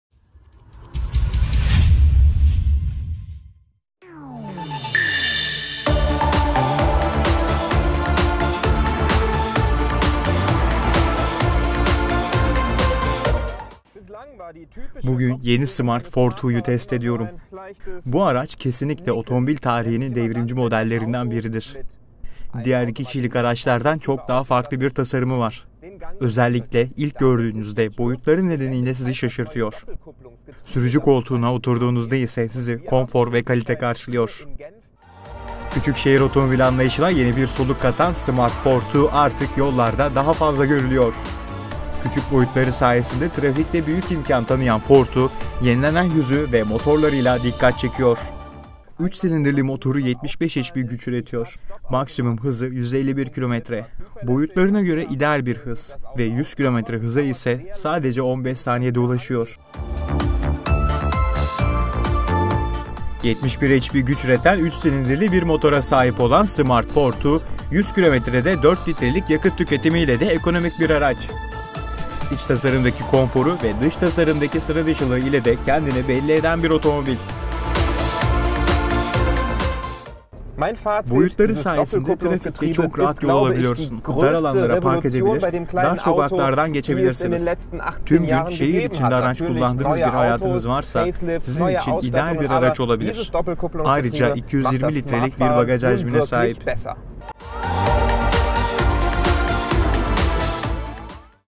Smart ForTwo 2015 Test Sürüşü
Son yılların en çok dikkat çeken modellerinden biri olan ve her geçen gün otomobil piyasasındaki yerini arttıran Smart ForTwo.